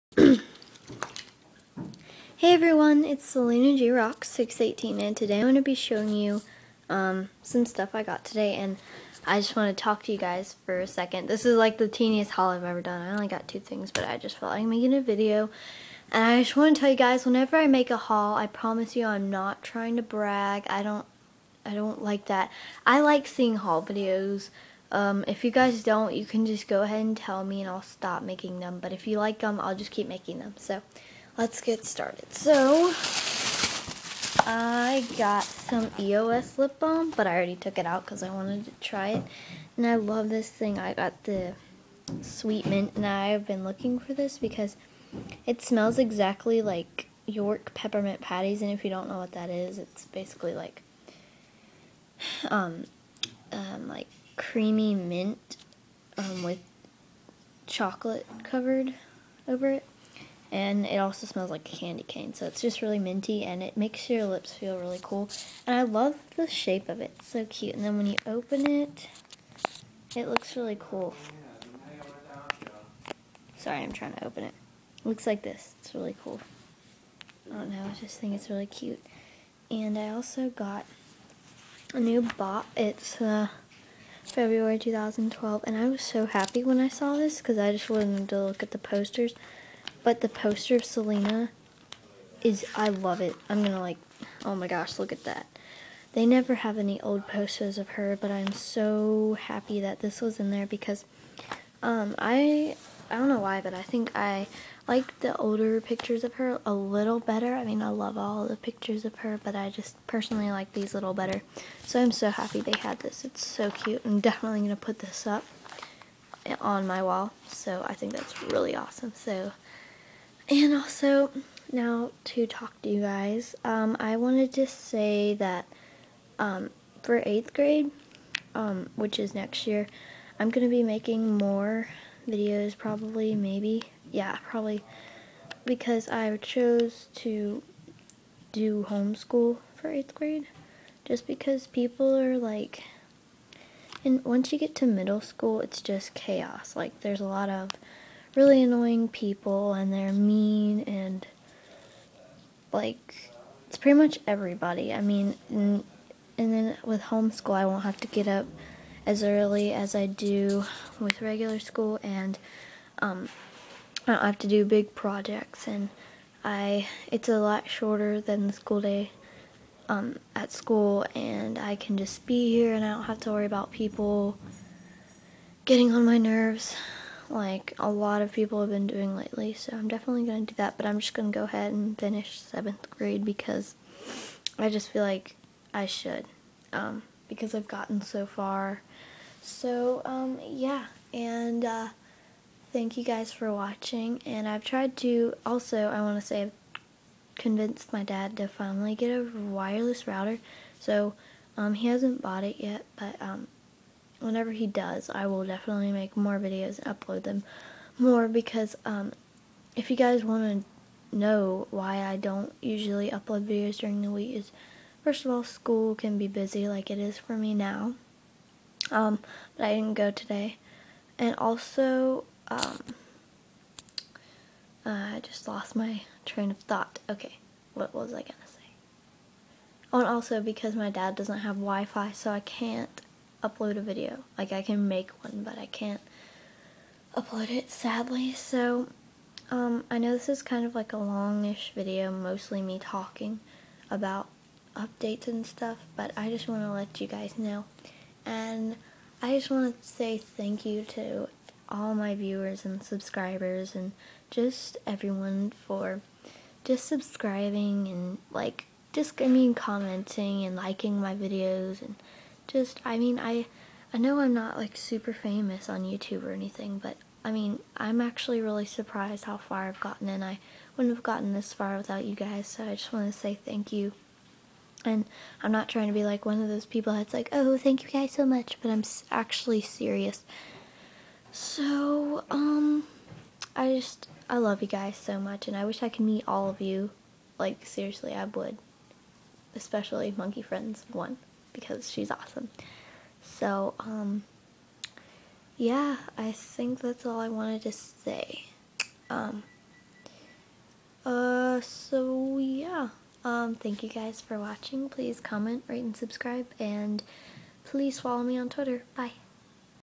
Mini Haul and Important Update
I love you guys sooo much and I want you to know that, as of now I am in the process of getting my dad to get wifi soo hopefully that will be soon. *Just a little reminder these items were purchased by ME with my own money. (: